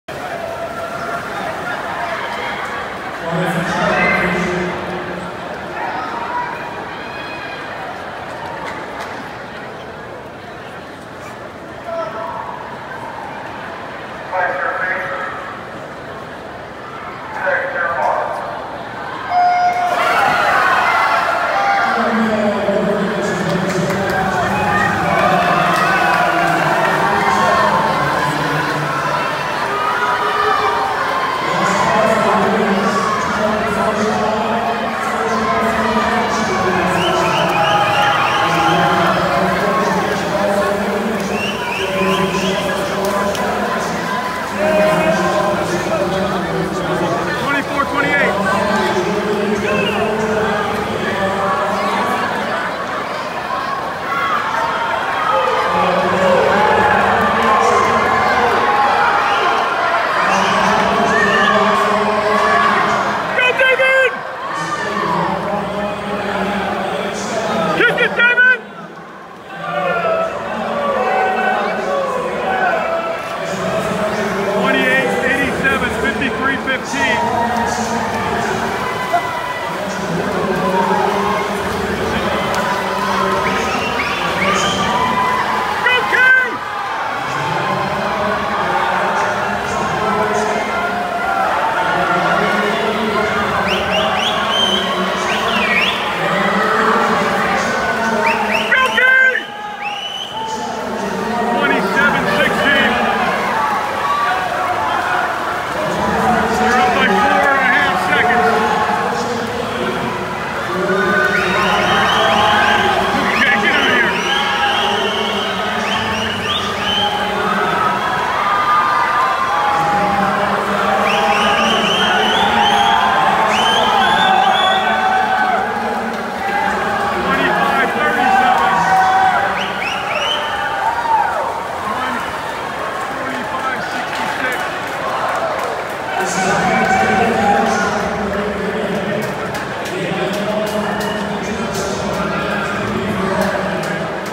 Obama boys 200 medley relay
Doing an 8 second drop from seed time.
Sead, 24.25 back
Noah, 28.89 breast, 53.15
David, 27.16 fly, 1:20.31
K Alagar, 25.37, free, 1:45.65

Points but no podium.